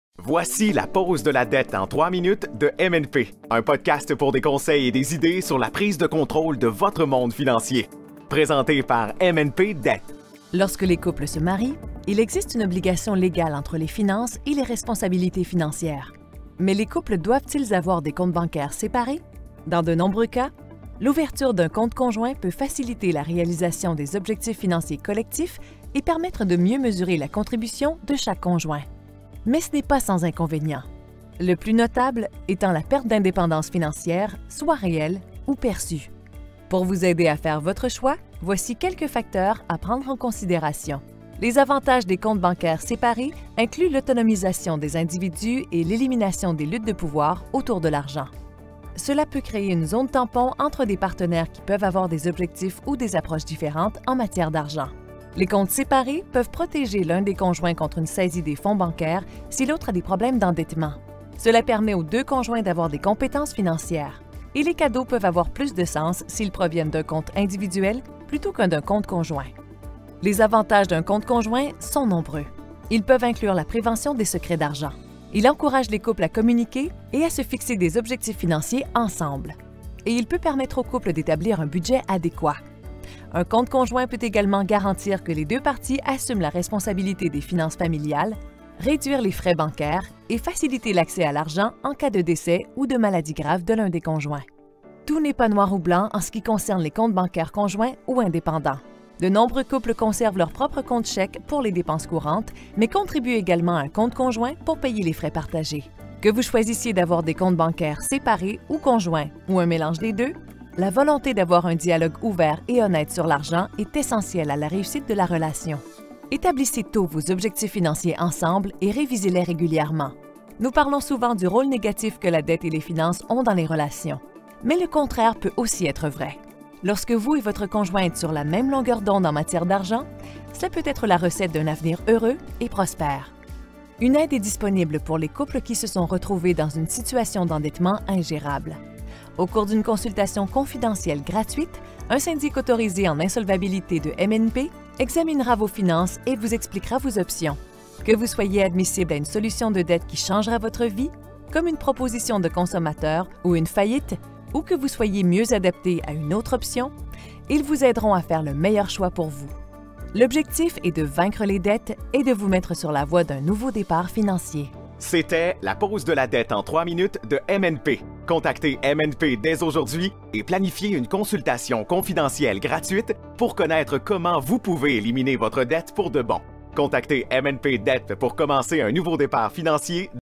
Les couples devraient-ils avoir des comptes bancaires distincts? (Parlons dettes en 3 minutes)
Lorsque les couples se marient, il existe une obligation légale entre les finances et les responsabilités financières. Mais les couples doivent-ils avoir des comptes bancaires séparés ? Dans de nombreux cas, l’ouverture d'un compte conjoint peut faciliter la réalisation des objectifs financiers collectifs et permettre de mieux mesurer la contribution de chaque conjoint. Mais ce n'est pas sans inconvénient - le plus notable étant la perte d'indépendance financière soitréelle ou perçue.

Pour briser le cycle de l’endettement, demandez une consultation confidentielle gratuite avec un syndic autorisé en insolvabilité de MNP.